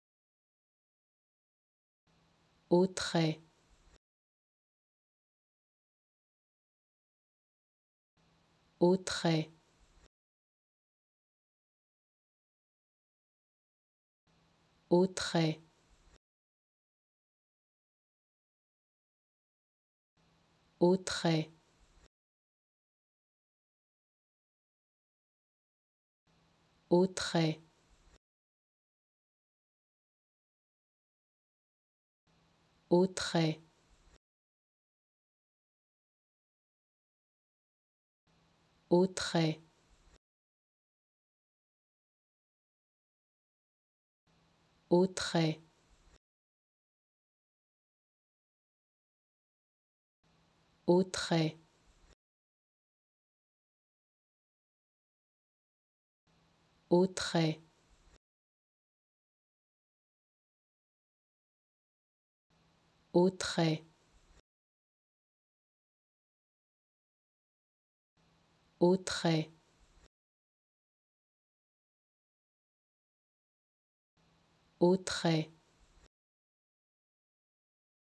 How to Pronounce ''Autrey'' Correctly in French
Learn how to say and properly pronounce ''Autrey'' in French with this free pronunciation tutorial.